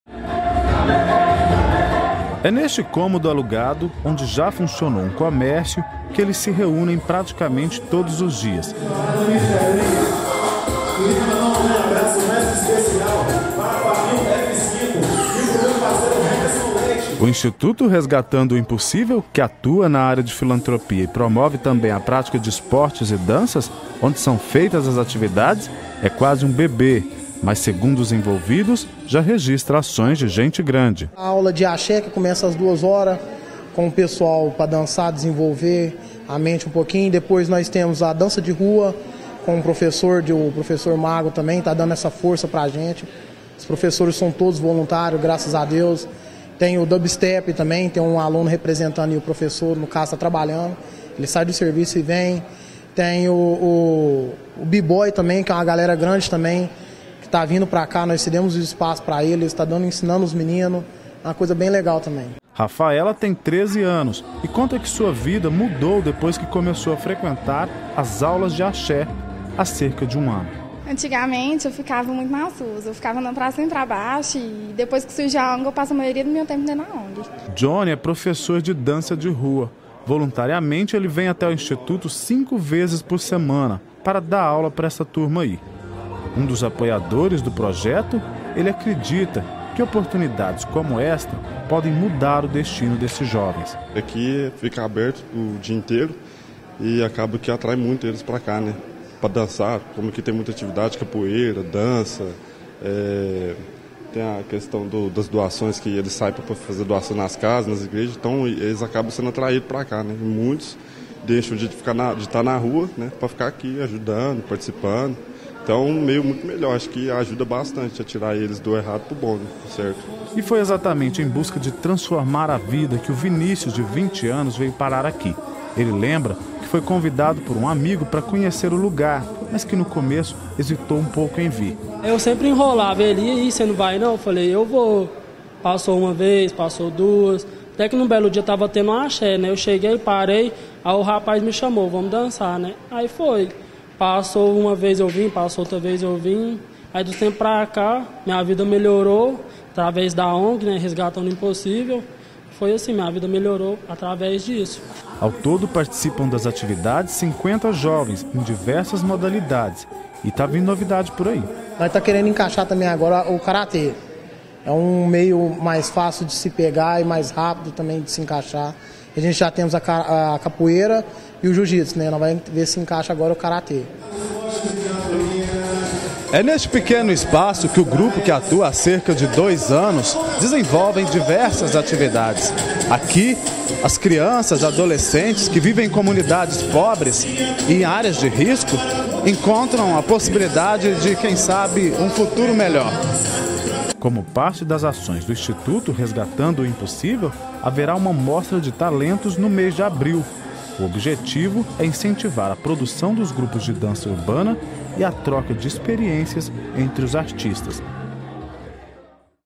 Mostra Hip Hop - 22/02/2016
Para incentivar a produção de dança urbana e promover o intercâmbio entre os artistas, um instituto de Uberlândia promove uma mostra de talentos. Você vai conhecer agora, apenas uma das ações sociais do grupo que atua na cidade.